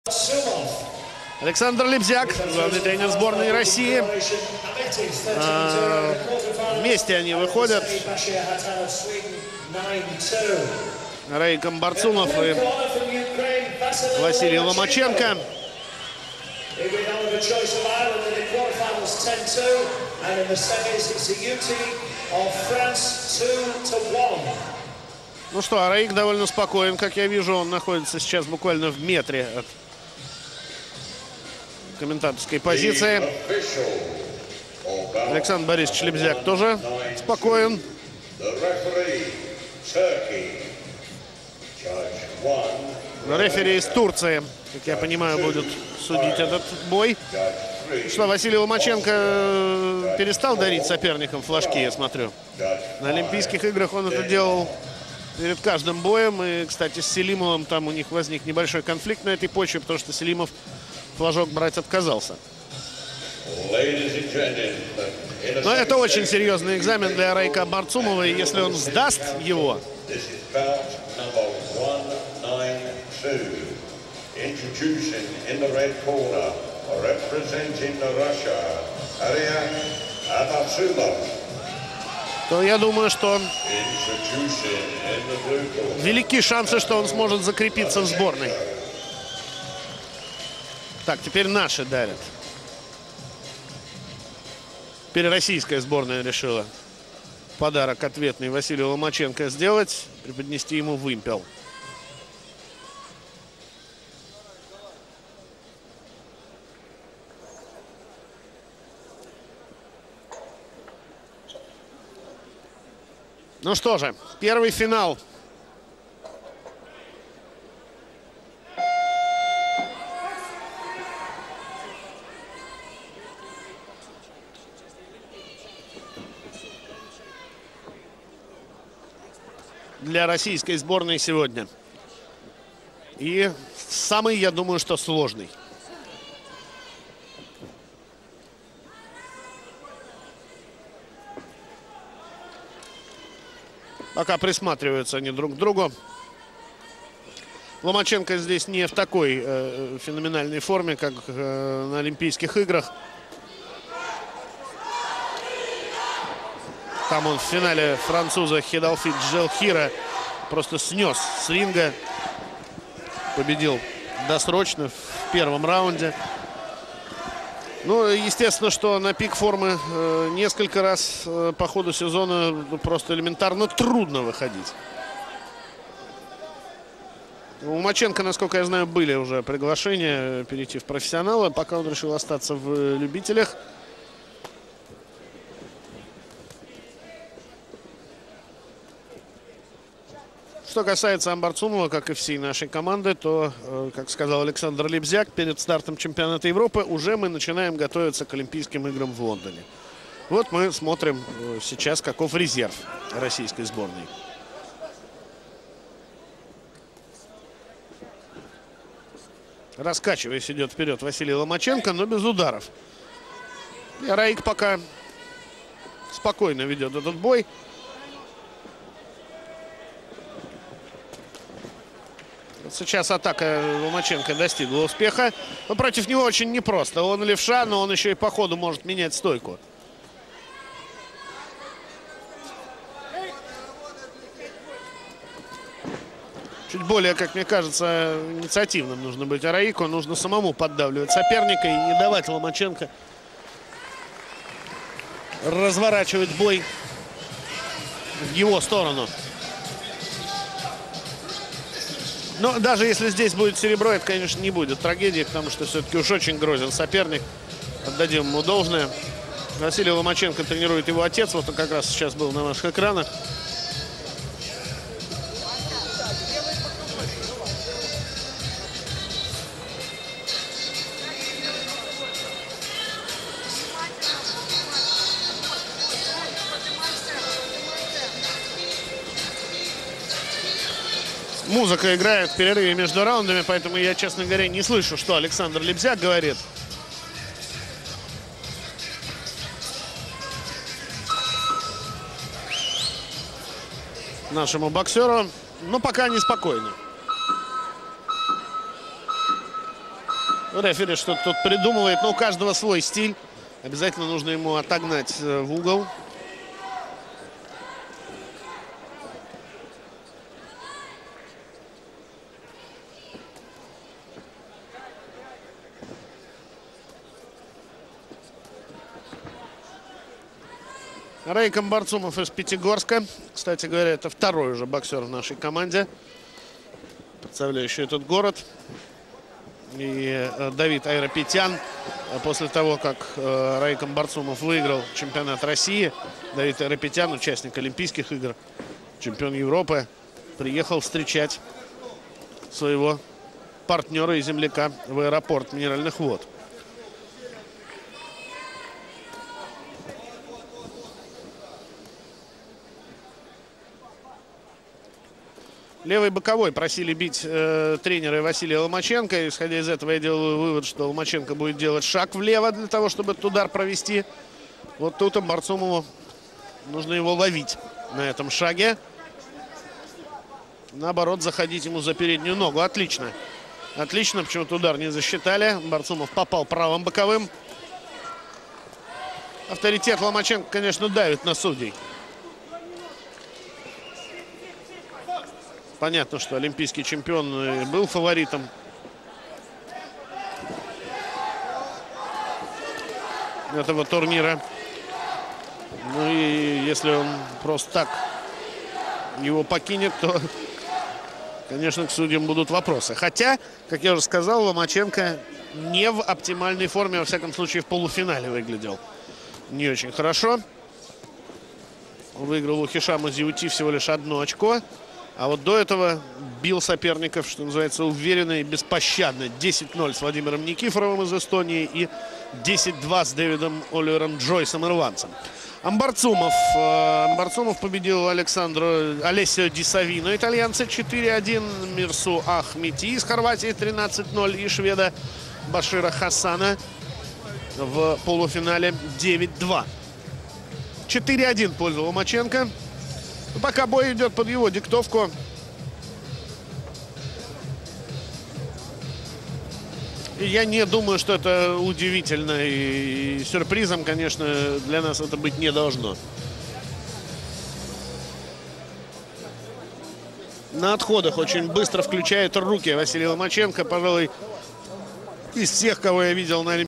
Vasyl Lomachenko vs Araik Ambartsumov 2008 European Amateur Boxing Championships FINAL Part 1
Featherweight(- 57 kg)FINAL 2008 European Amateur Boxing Championships Part 1 Vasyl Lomachenko (Ukraine) vs Araik Ambartsumov (Russia)